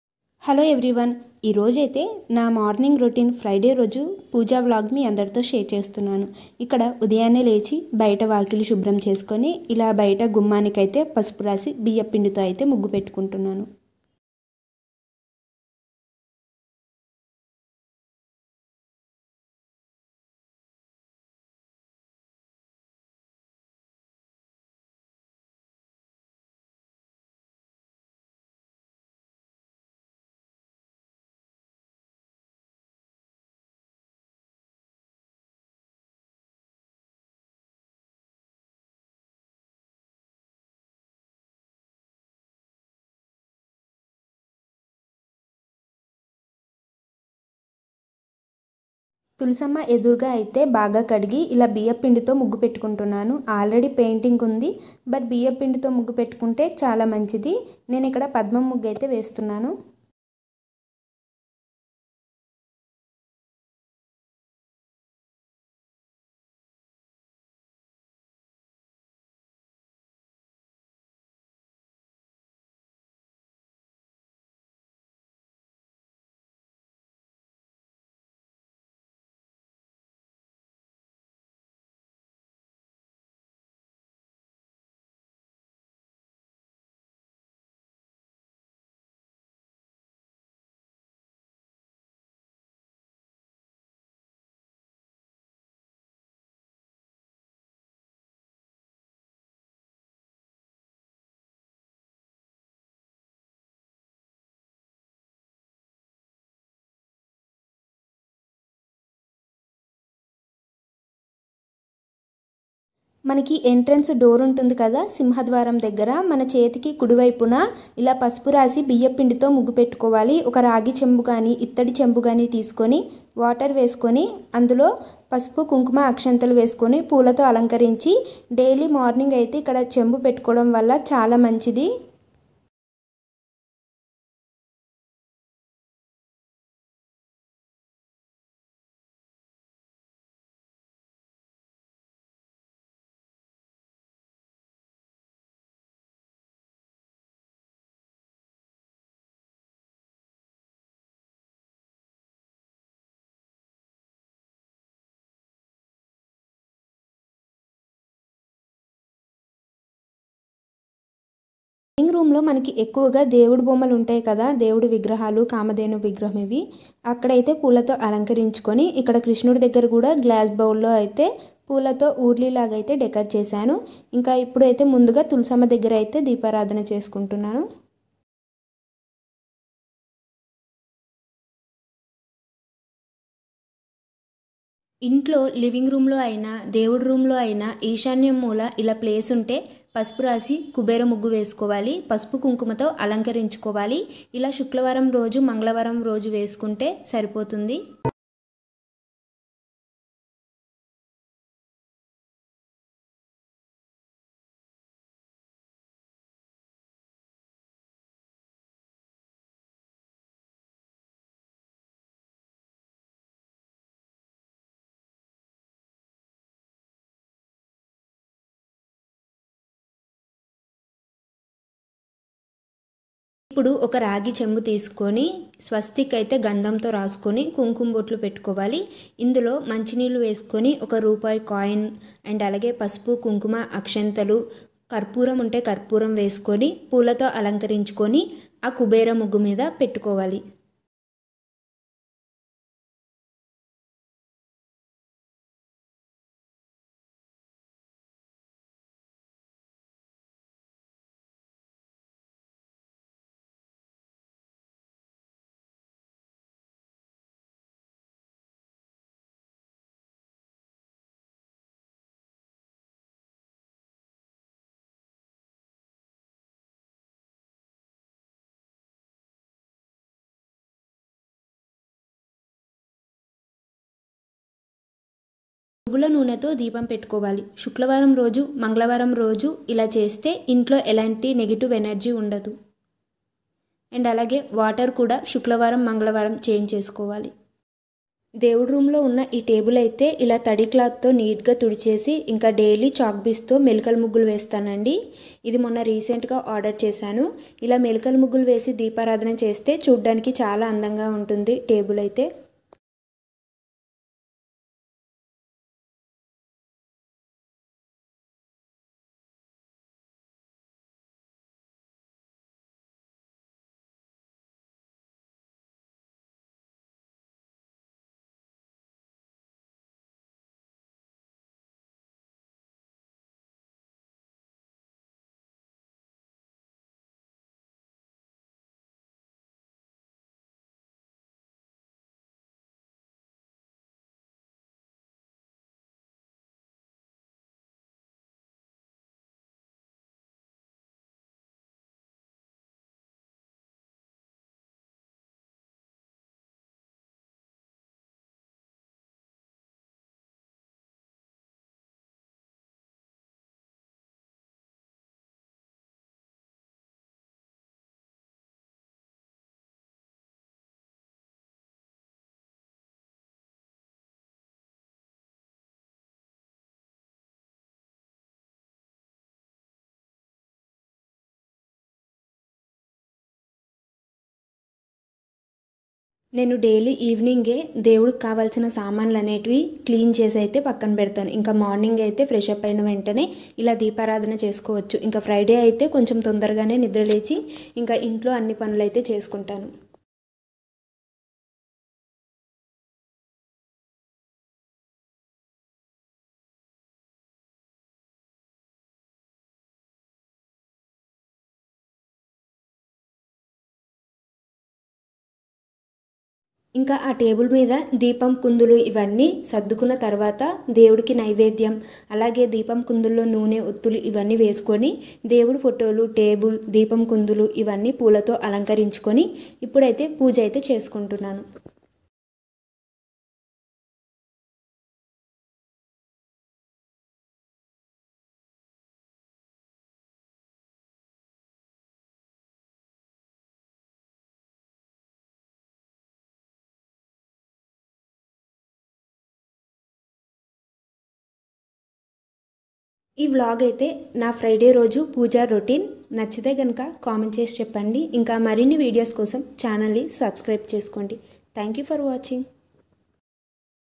శుక్రవారం ఉదయం 4 గంటలకు నేను చేసుకునే పూజా విధానం,అలాగే పనులుcleaningmotivation
telugu vlogs
vlogs in telugu
daily vlog
be more productive
housewife routines
telugu vlogs America